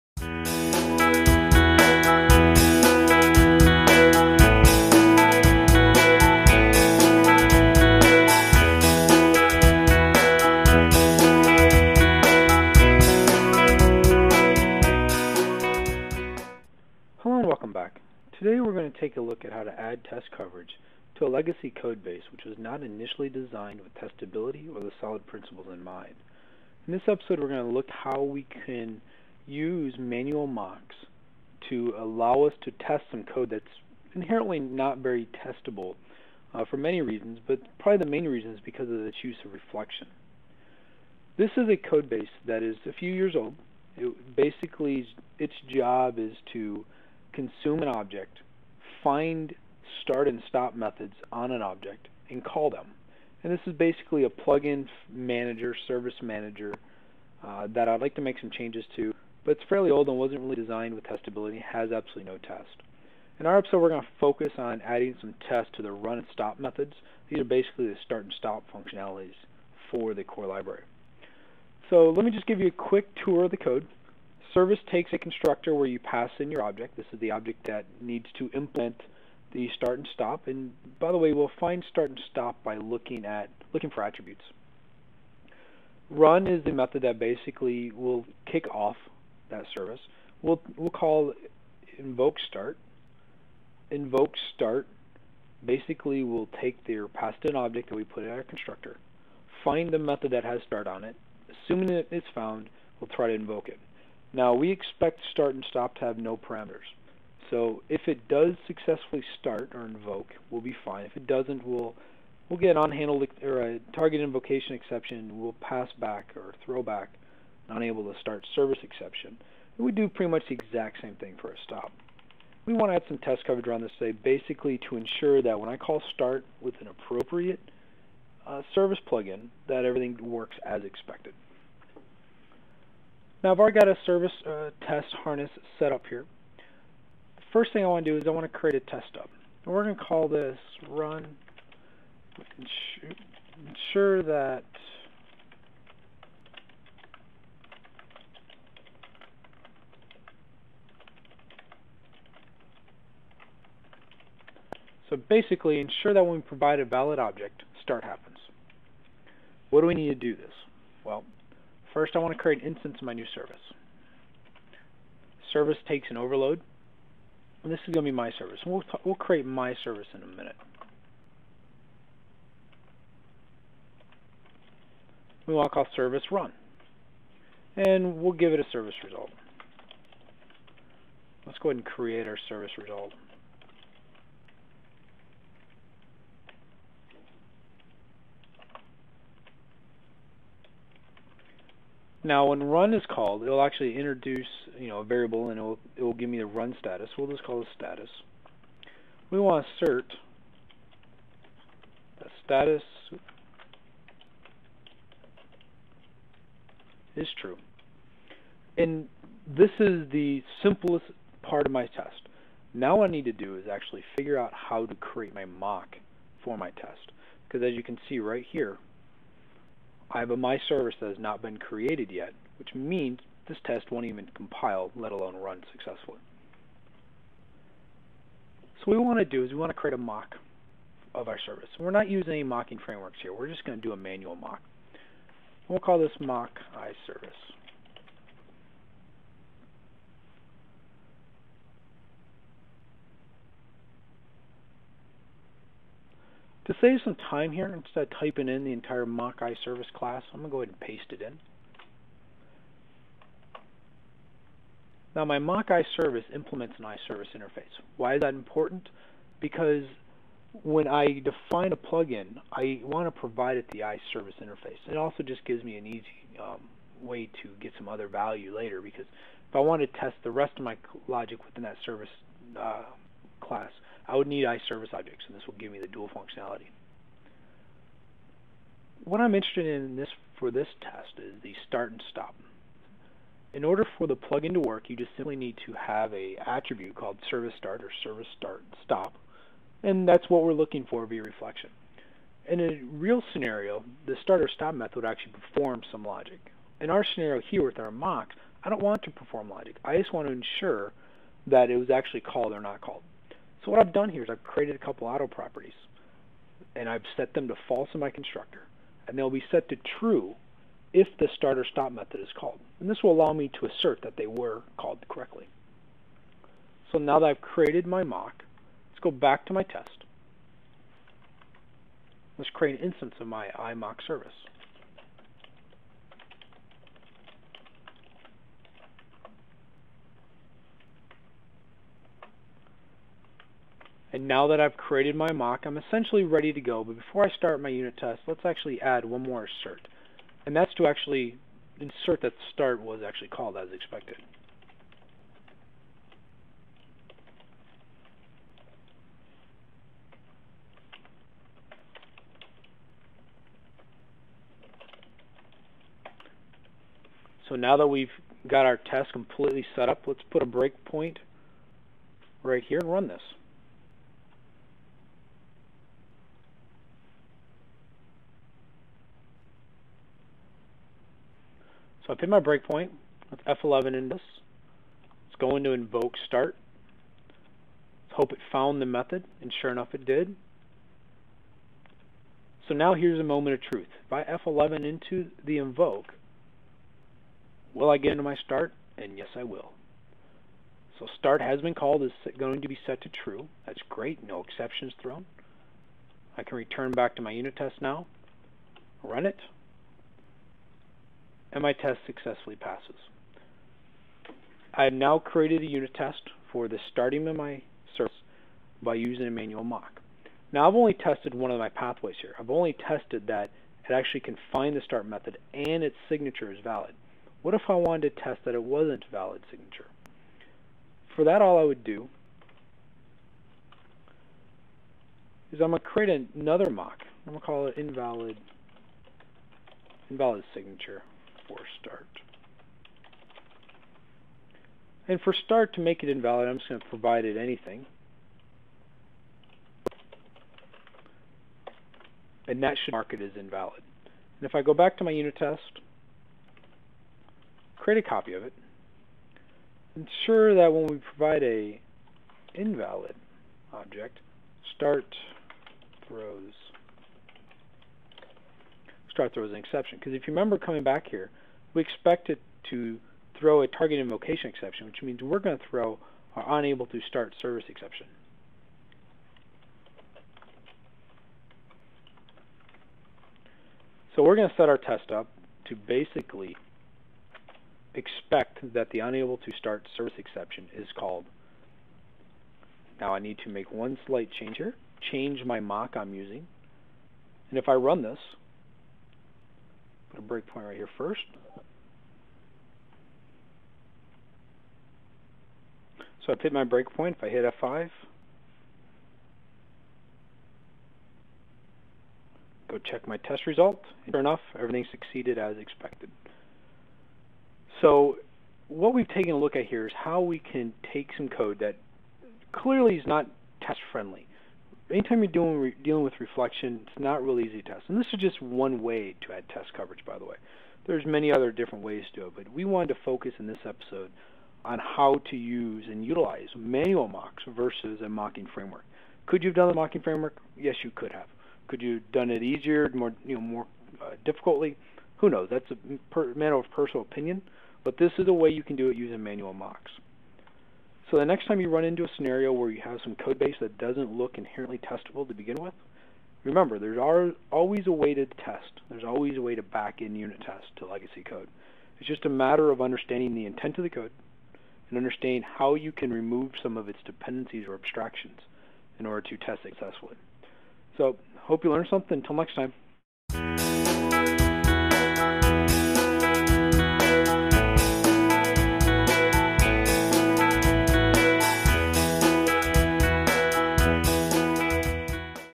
Using Manual Mocks for Testing
Episode #164

In this episode we are going to take a look at how to use manual mocks for testing.
Often times when creating unit tests we need to work in isolation in order to cover the paths we are attempting to test.

When we want to test in isolation you can use a testing technique where you mock out your dependencies.

When using Mocks you can either do it manually (what we are looking at) or you can use a mocking framework like Rhino Mocks. Either way you achieve the same results.

Author: Derik Whittaker
Level: Intermediate
Recorded on: 2/23/2010

programming